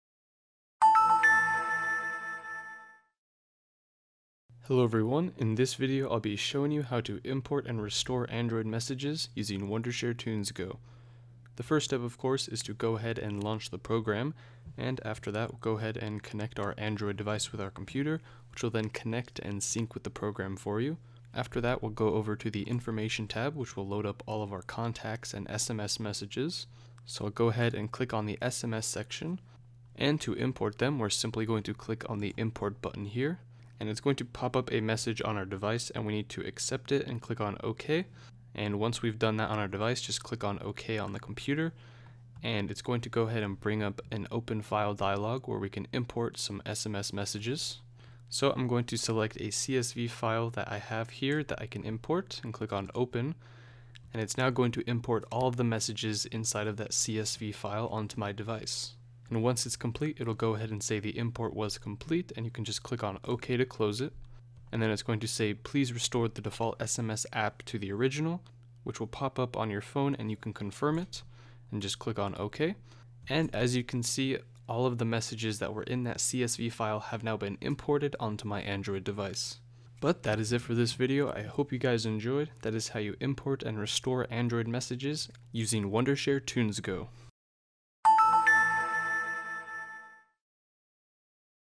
How to Import and Restore Android SMS
Wondershare TunesGo works like a desktop SMS assistant, helping to import and restore all SMS to your Android phone from computer CSV or HTML backup.

How to Import and Restore Android SMS from Backup Files on Computer with TunesGo: